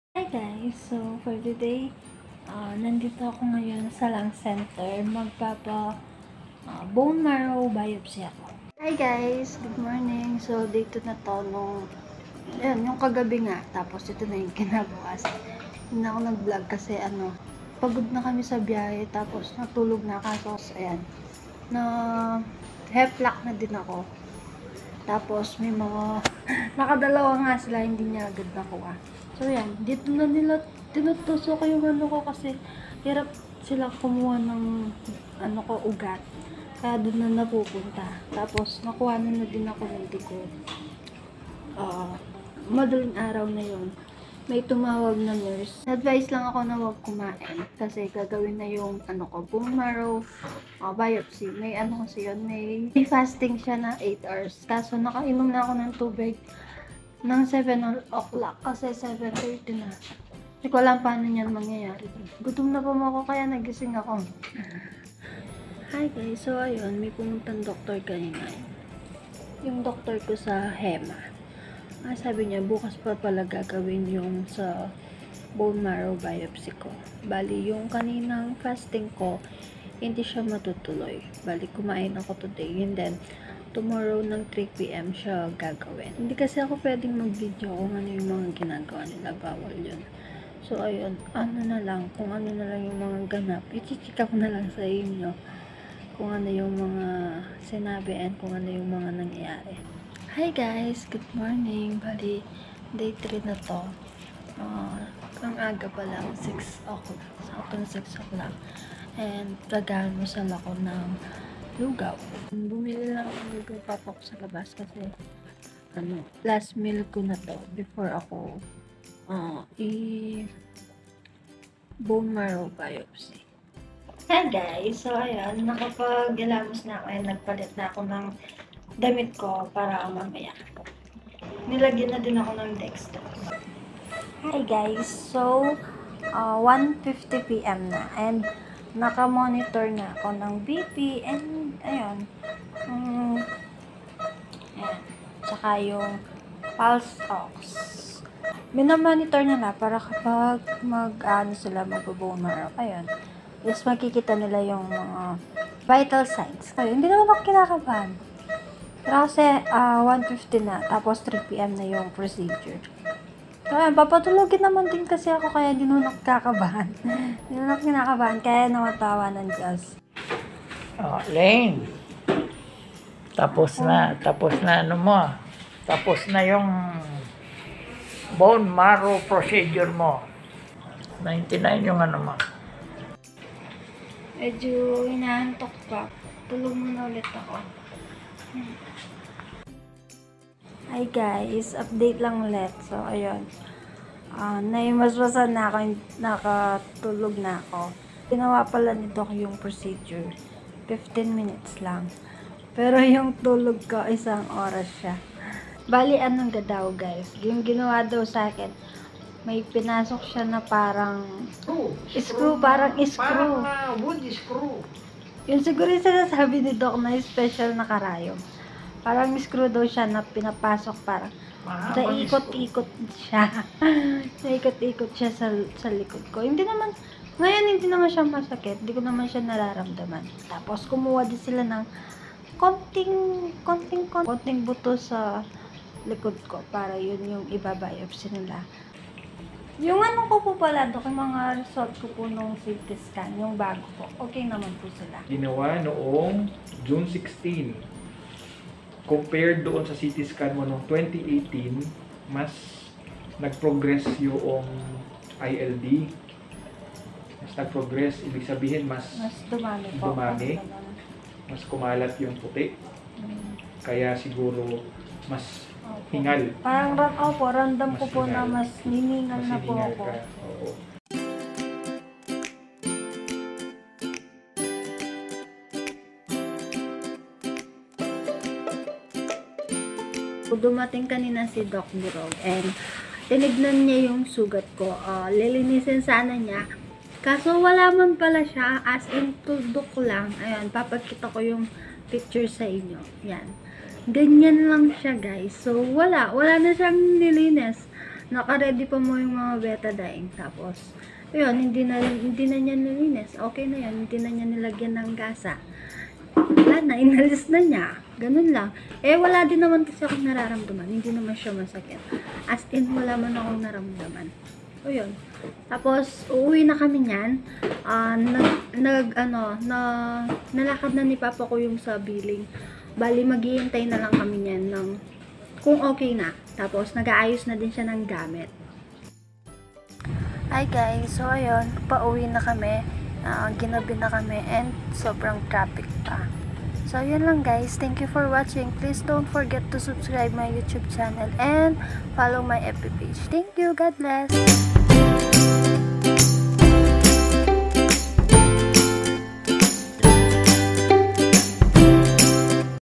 BONE MARROW BIOPSY JOURNEY | Marilane Quiambao
Dr. Villaroman
Dr. Maltbie
Dr. Francisco
Dr. JP Bulaclac
Dr.Burog
Dr.Del Rosario
Dr.Balaoing
Ma'am Kute
Ma'am Donna
Ma'am Doris

For helping my hospital bills 💞